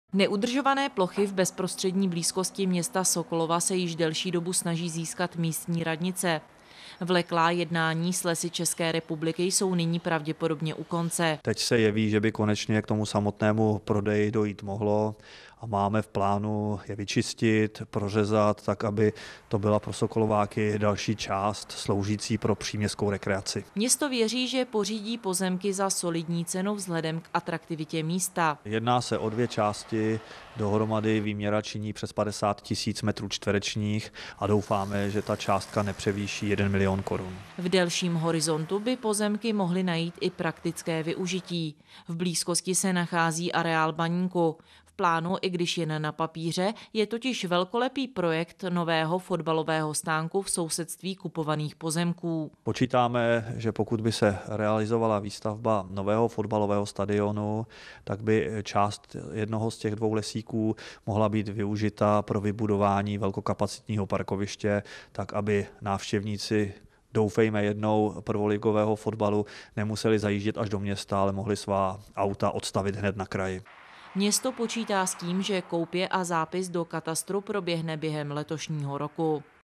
2010 – Sokolov: Město chce koupit lesík nad zimním stadionem (4015) (TV Západ)
Neudržované plochy v bezprostřední blízkosti města Sokolova se již delší dobu snaží získat místní radnice. Vleklá jednání s Lesy České republiky jsou nyní pravděpodobně u konce.

"Teď se jeví, že by konečně k tomu samotnému prodeji dojít mohlo, a máme v plánu je vyčistit, prořezat tak, aby to byla pro Sokolováky další část sloužící pro příměstskou rekreaci," vysvětlil záměry starosta Karel Jakobec.

Město věří, že pořídí pozemky za solidní cenu vzhledem k atraktivitě místa.

"Jedná se o dvě části, dohromady výměra činí přes 50 tisíc metrů čtverečních a doufáme, že částka nepřevýší jeden milion korun," upřesnil Jakobec.

V delším horizontu by pozemky mohly najít i praktické využití. V blízkosti se nachází areál Baníku. V plánu, i když jen na papíře, je totiž velkolepý projekt nového fotbalového stánku v sousedství kupovaných pozemků.

"Počítáme, že pokud by se realizovala výstavba nového fotbalového stadionu, tak by část jednoho z těch dvou lesíků mohla být využita pro vybudování velkokapacitního parkoviště tak, aby návštěvníci, doufejme jednou prvoligového fotbalu, nemuseli zajíždět až do města, ale mohli svá auta odstavit hned na kraji," prozradil Jakobec.

Město počítá s tím, že koupě a zápis do katastru proběhne během letošního roku.